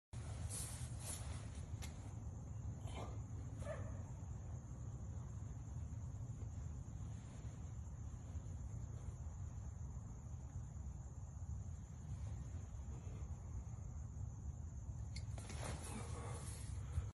Young Blood gots skills
@cotterbjj with the double-palmed kettlebell windmill